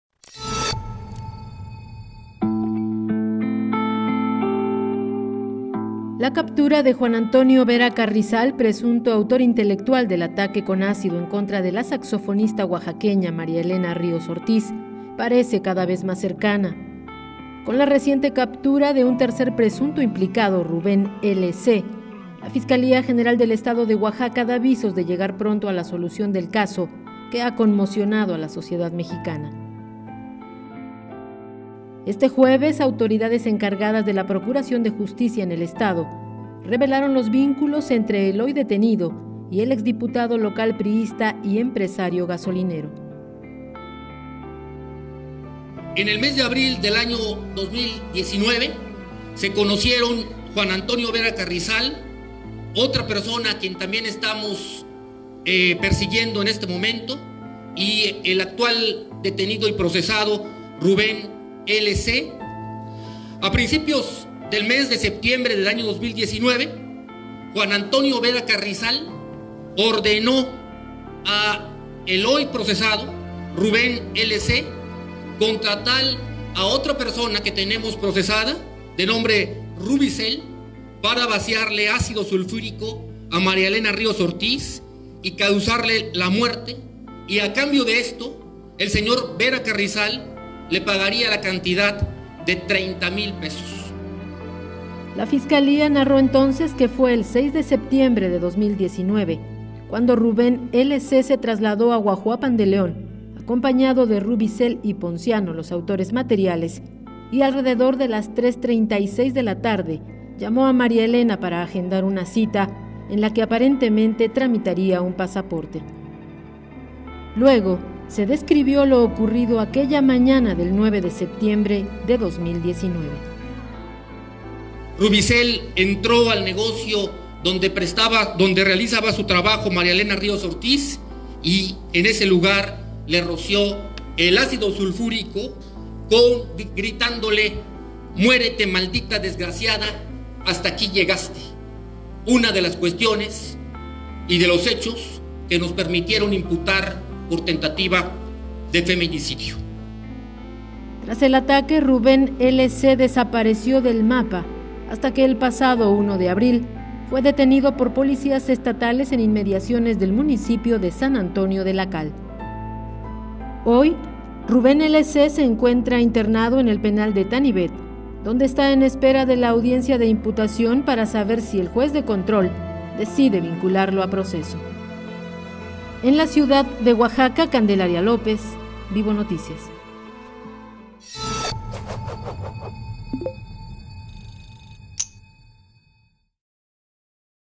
Capturan a tercer presunto implicado en el caso María Elena
La Fiscalía General del Estado dio a conocer la detención de Rubén L. C., quien presuntamente bajo las órdenes de Juan Antonio Vera Carrizal, organizó el ataque con ácido en contra de la saxofonista oaxaqueña, María Elena Ríos Ortiz.